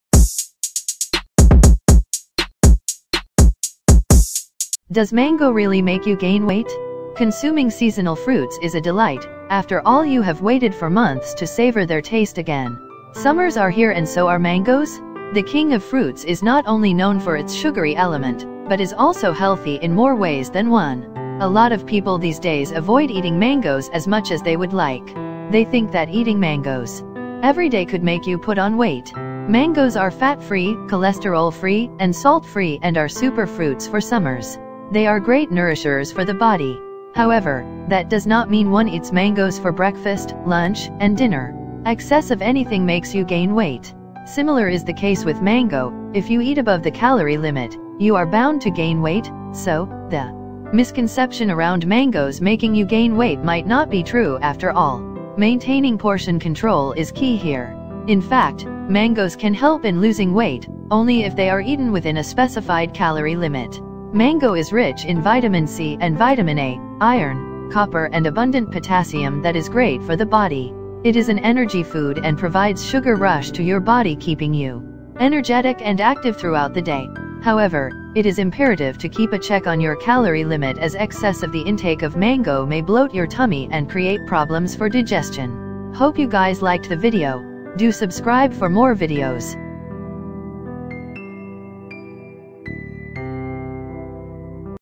Does Eating Mangoes Make you gain weight ? Mangoe benefits
Mangoes are very beneficial for our health. It contain many nutrients that helps your body. To know if eating mangoes make you gain weight. Watch the video

FLAIR FUSION
FLAIR F.